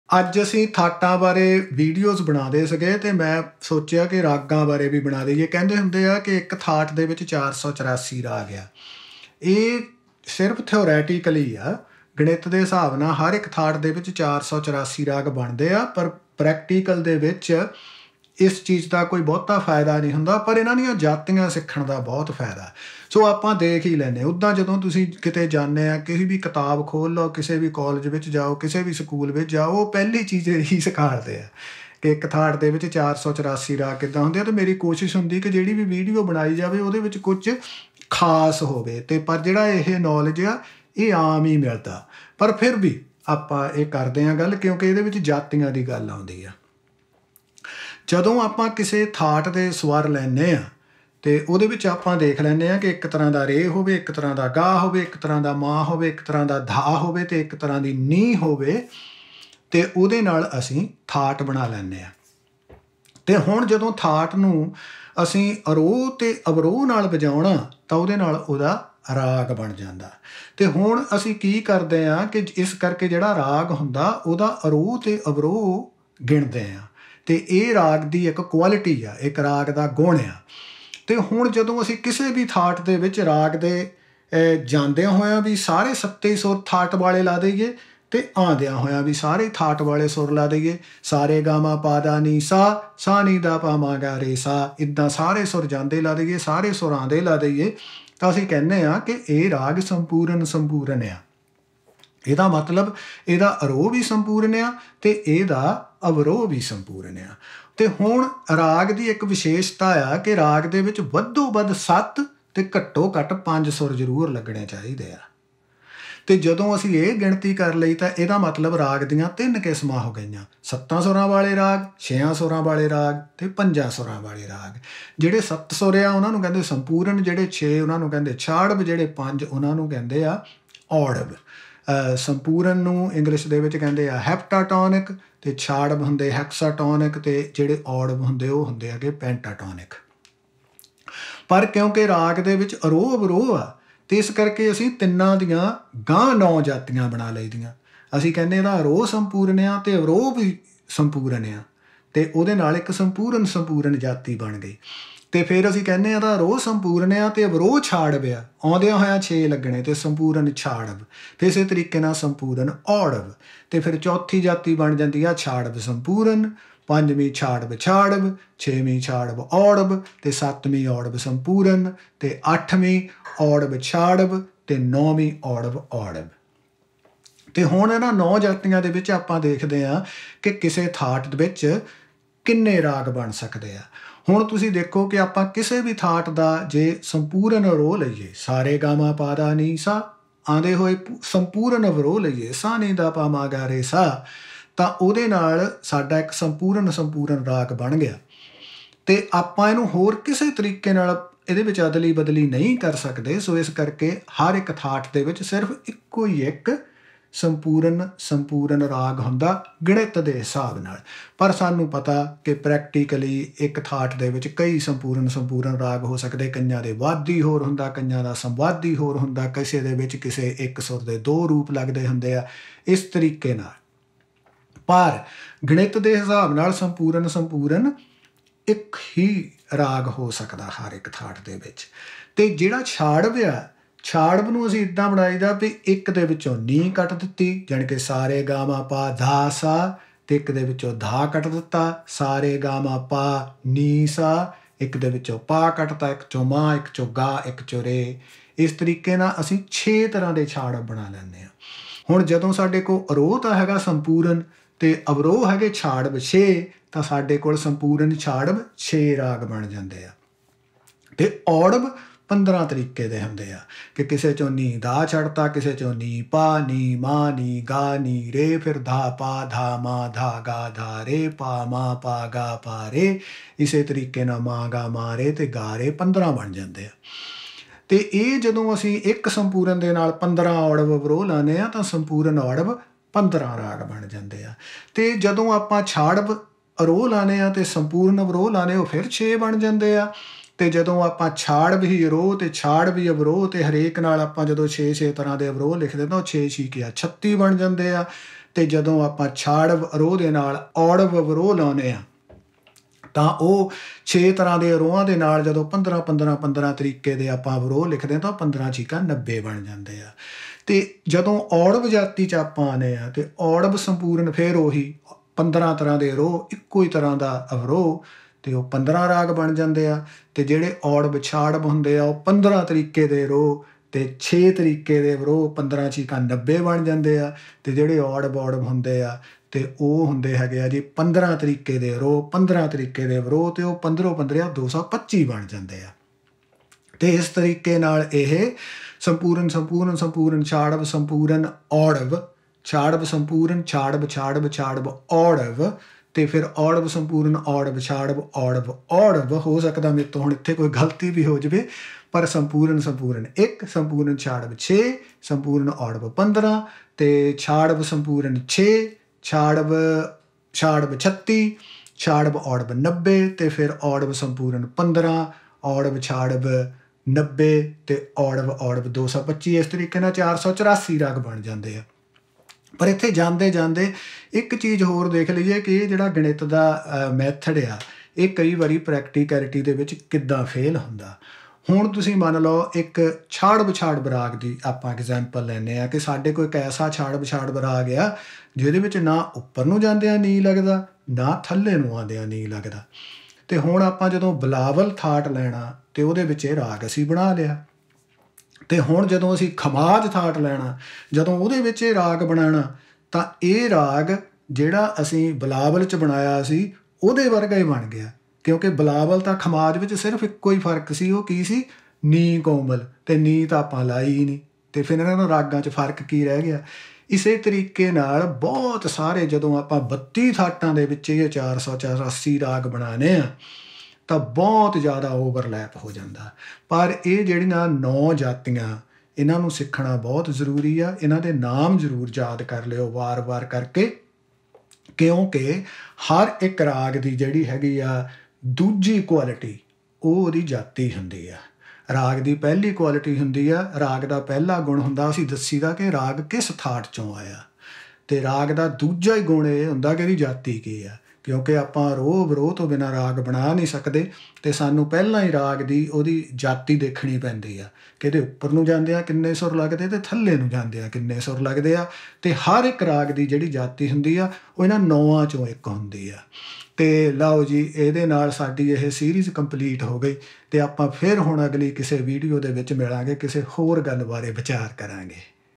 484 Ragas of a Thaat - Sangtar
484 Ragas of a Thaat by Sangtar

How to create Ragas from a Thaat? This tutorial is about the most comprehensive and easiest method to achieve this seemingly high number of Ragas from a parent scale.

Connect with Sangtar